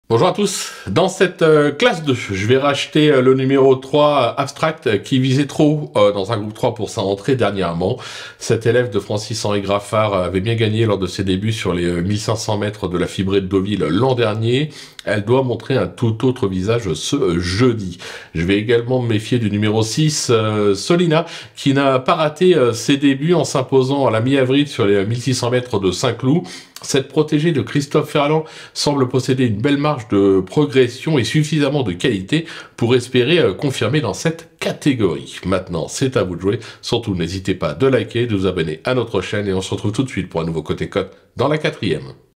PRONOSTICS COURSES PARISLONGCHAMP (R1-C3) | 09/05/2024 | COTE & COTE GENY COURSES AVEC GENYBET
Retrouvez nos pronostics pour le quinté et les courses de la réunion 1 de ce jeudi 9 mai 2024 sur l’hippodrome de ParisLongchamp dans le Cote & Cote de Geny Courses en partenariat avec Genybet.
Des bases en bétons, des outasiders séduisants, des coups de poker osés, des tocards repérés … Suivre les pronos de Philippe Thévenon, c’est l’assurance d’avoir toujours la Cote & Cote !